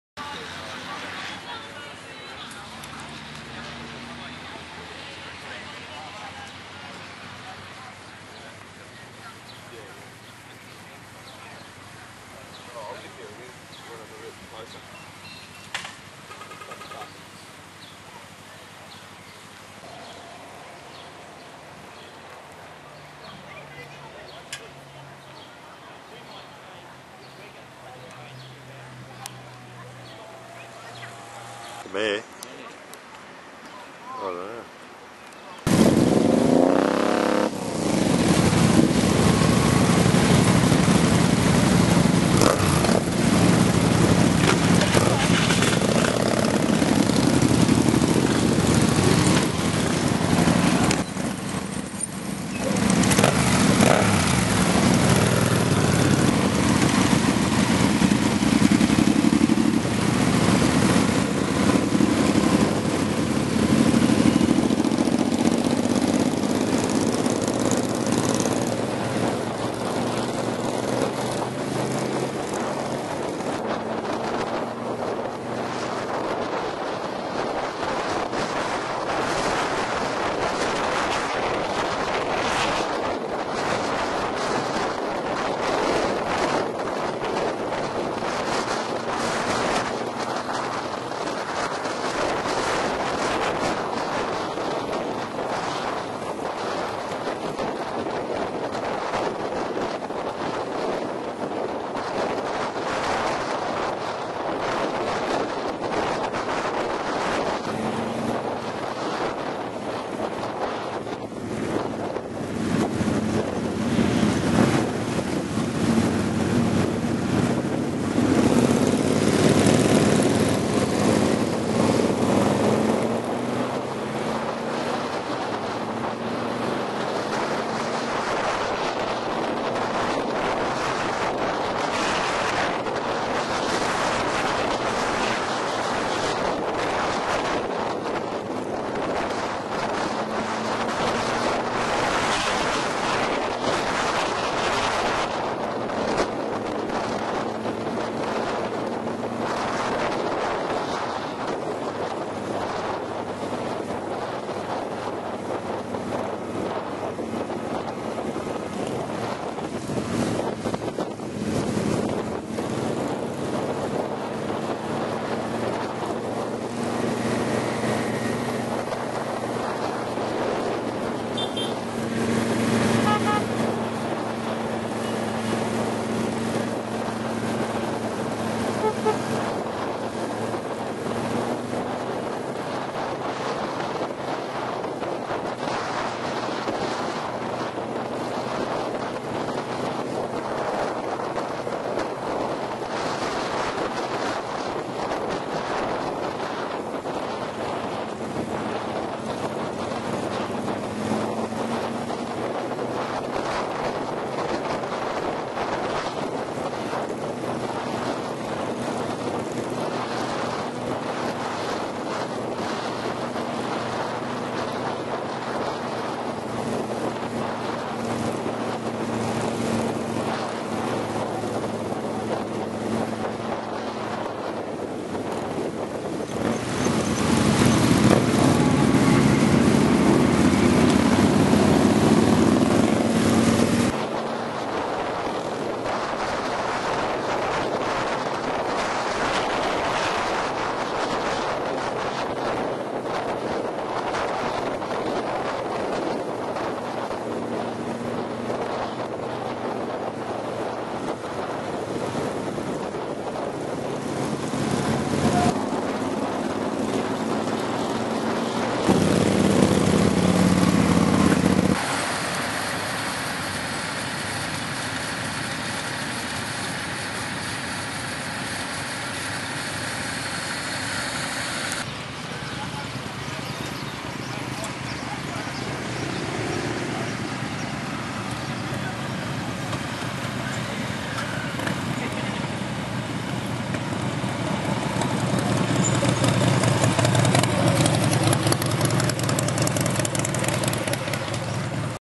2010 wagga wagga toy run (part 3 of 4)
highlights of wagga wagga toll toy run december 2010